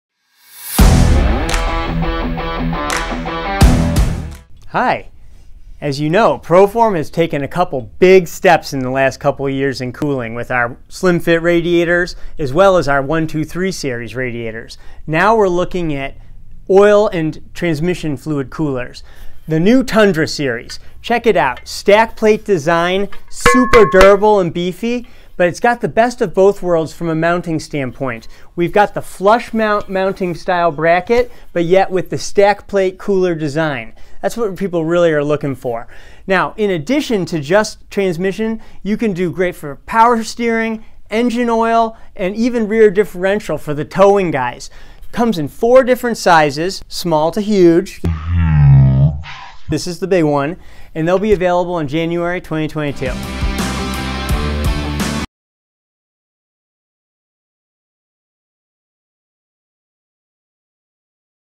SEMA 2021 Tundra Series Oil Transmission Coolers New Product Video
You don’t need an advanced degree in mathematics to know that cooler fluid equals optimum performance with longer engine and component life. Tundra Series Universal Transmission Oil Coolers keep oil temperatures where they need to be for peak performance! The stacked plate and fin design is up to 29.387% more efficient than same size tube and fin coolers. That’s important when every 20°F decrease in temperature can double the oil life and increase transmission component durability.

          Features:
            • For use with Power Steering, Engine Oil, or Rear Differential Fluids
            • Multi-Point, Universal Stacked-Plate Oil Cooler Design Is Compact,
                Great for Narrow Spaces
            • Prevents Engine Coolant & Transmission Fluids from Accidently
                Mixing, Avoiding Expensive Repairs
            • Low Pressure-Drop Design with Less Oiling-System Restriction

 Part Number                       Cooling Rows:      Overall Height (in.):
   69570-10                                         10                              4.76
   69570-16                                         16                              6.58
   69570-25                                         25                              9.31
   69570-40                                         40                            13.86